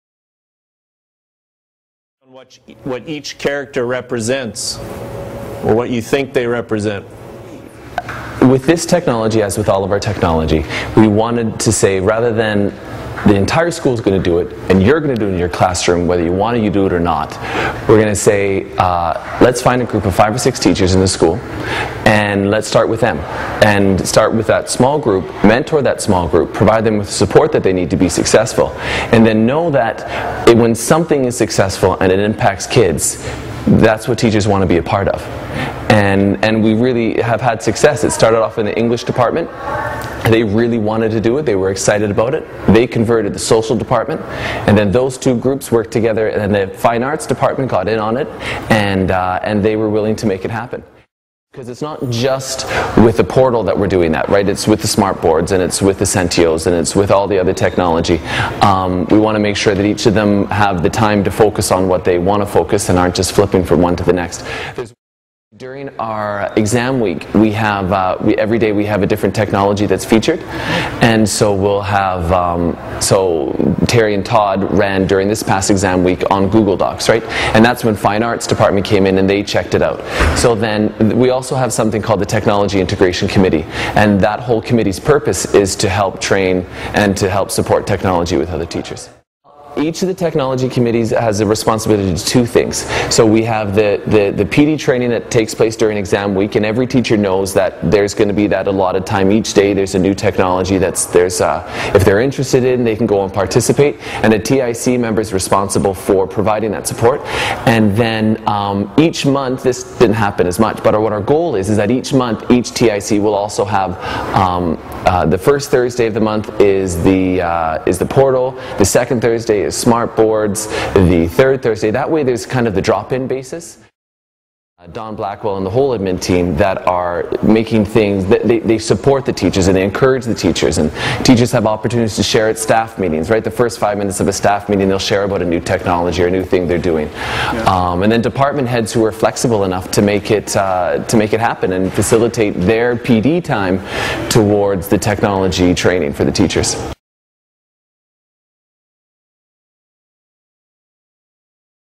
Technology Integration at Queen Elizabeth High School
Terry Godwaldt talks about how Queen E High School integrates technology using a distributed approach. Specific technologies, such as Google Apps (a.k.a. the Student Portal), Smart Boards, or Student-Response systems are rolled out department by department, teacher by teacher, depending on interest.